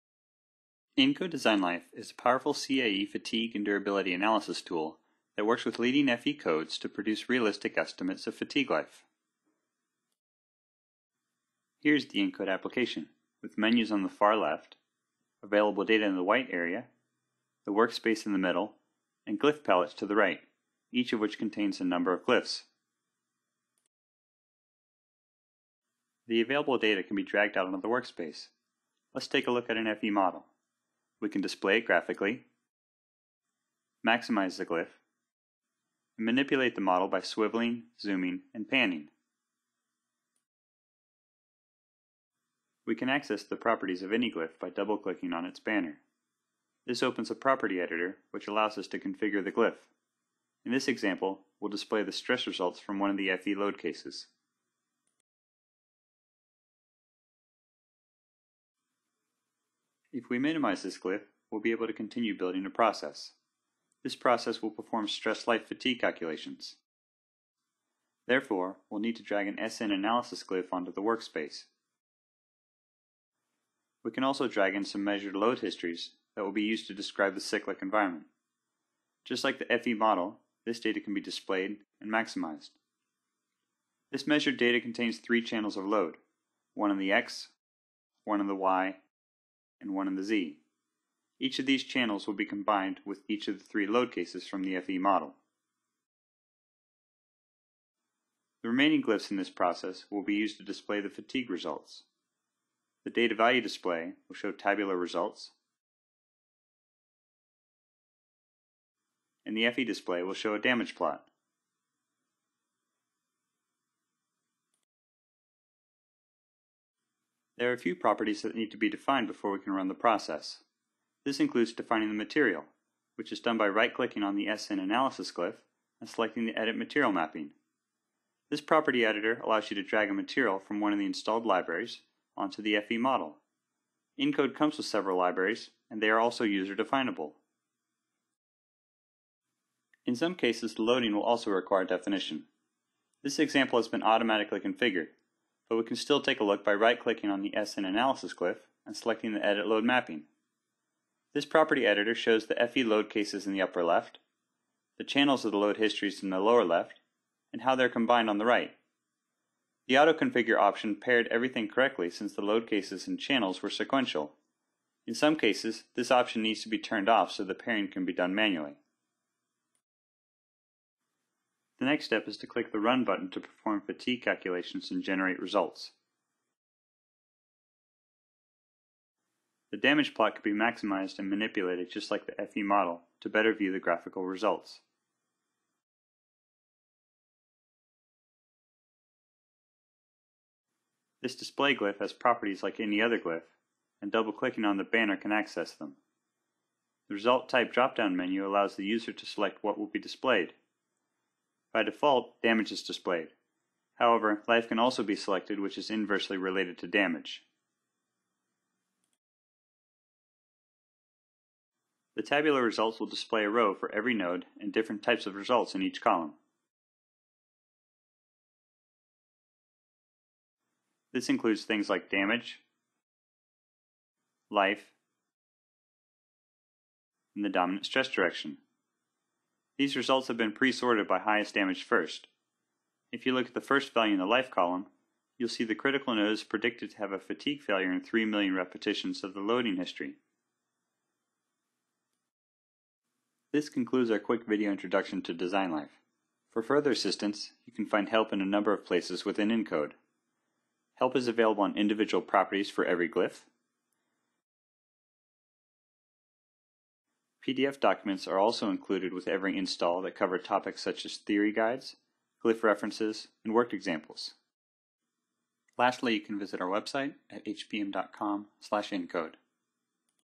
Getting Started with nCode DesignLife
nCode DesignLife is CAE-based software solution for fatigue and durability analysis. DesignLife works with all leading finite element (FE) codes and produces realistic predictions of fatigue hotspots and fatigue life. DesignLife shares an environment with nCode GlyphWorks, a graphical test data processing software, to provide an unparalleled integration of test and CAE data.